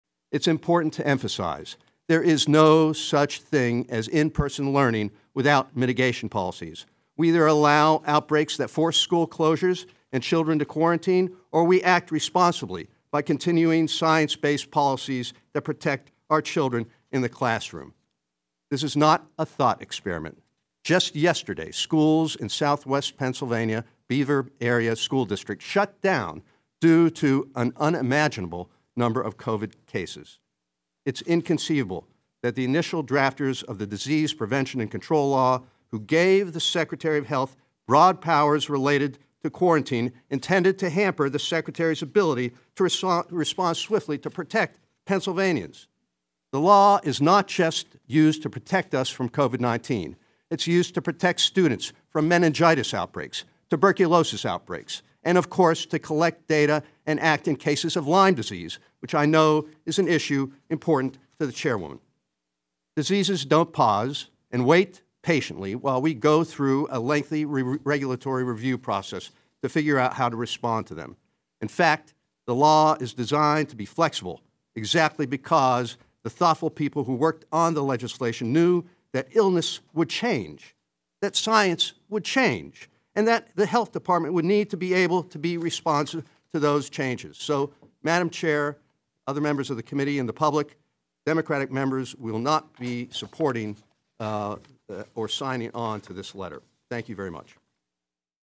Frankel Stands Against Limiting Health Secretary Powers
Pa. state Rep. Dan Frankel, Democratic Chair of the House Health Committee, spoke against an effort by the Republican-controlled committee to limit the powers of the state Health Secretary to respond to a pandemic or future public health emergency.More